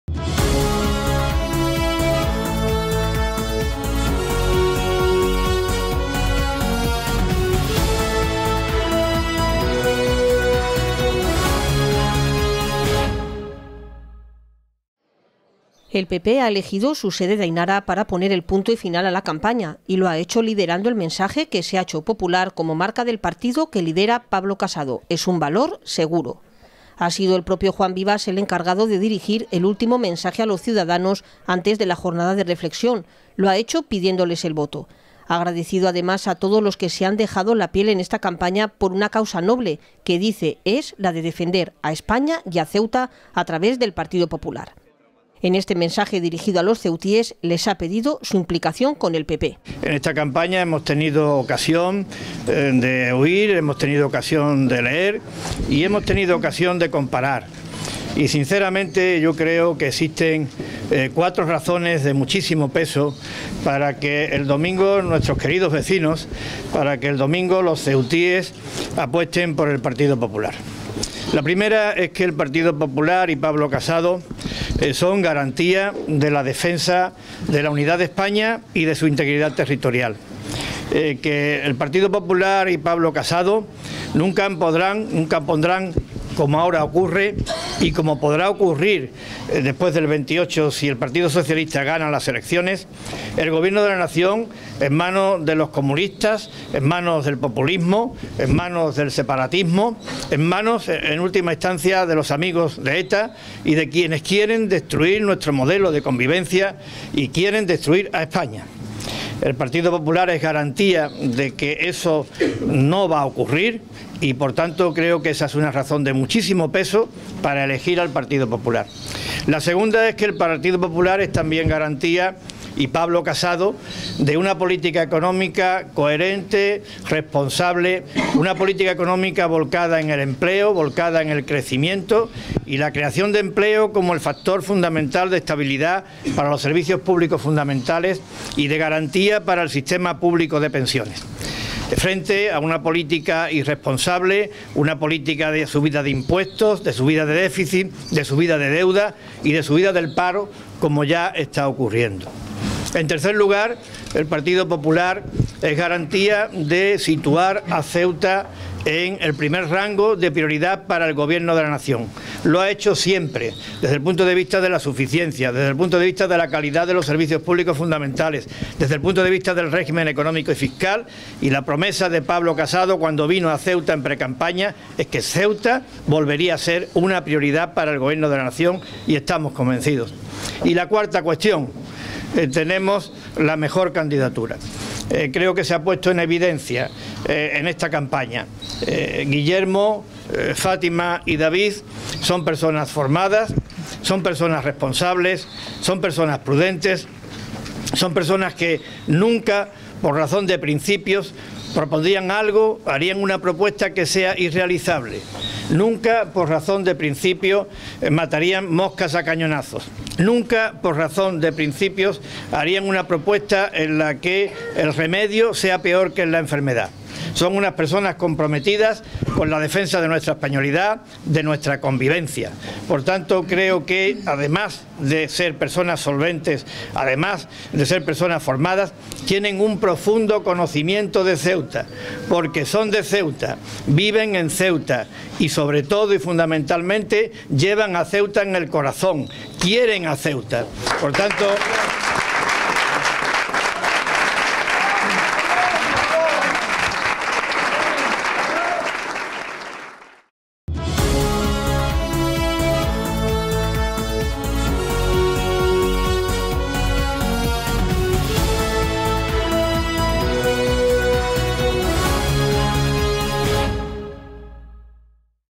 PP El Partido Popular pide el voto por una “causa noble”: defender a “Ceuta y a España”
El PP ha elegido su sede de Ainara para poner el punto y final a la campaña y lo ha hecho liderando el mensaje que se ha hecho popular como marca del partido que lidera Pablo Casado: es un valor seguro. Ha sido el propio Juan Vivas el encargado de dirigir el último mensaje a los ciudadanos antes de la jornada de reflexión. Lo ha hecho pidiéndoles el voto.